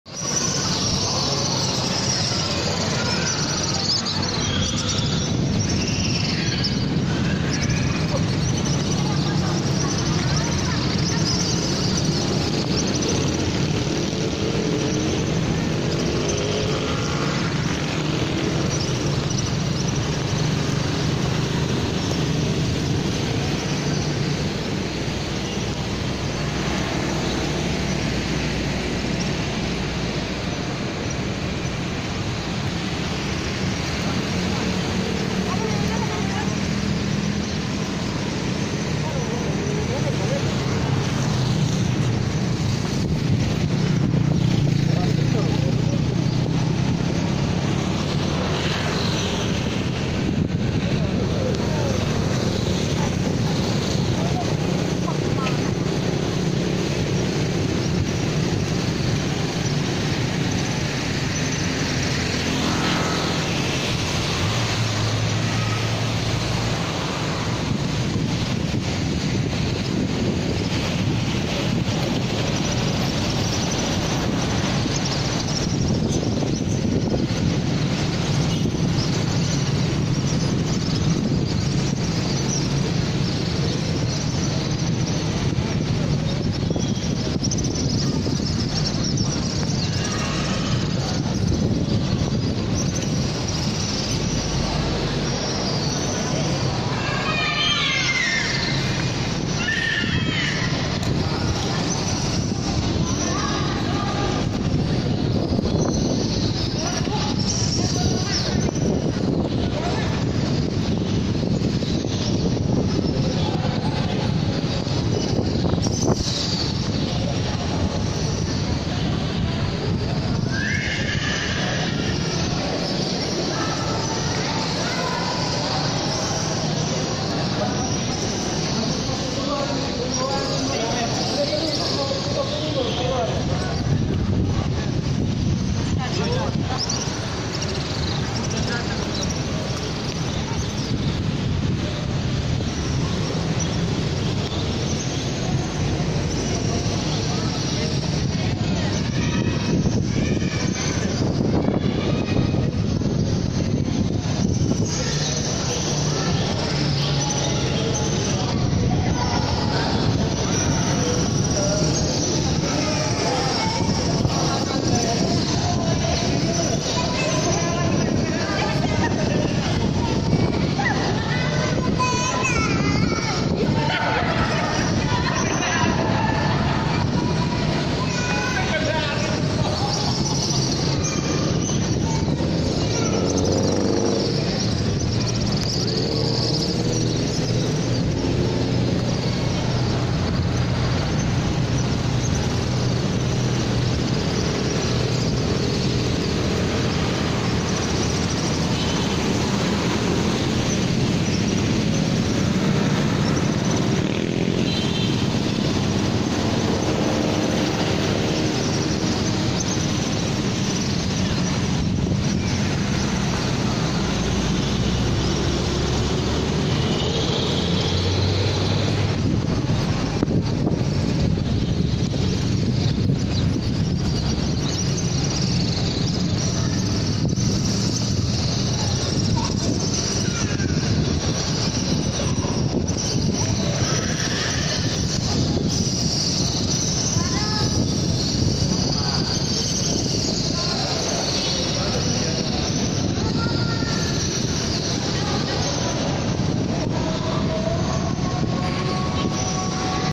Monumen Simpang Lima Gumul - Kediri
Alkhamdulillah video ini adalah upaya saya dalam mewujudkan rasa syukur saya kepada tuhan yang maha esa atas sebuah perjalanan jauh yang saya tempuh hingga sampai disini dengan selamat, kalau menurut pendapat saya : video lebih kuat dalam mengabadikan moment dibandingkan photo.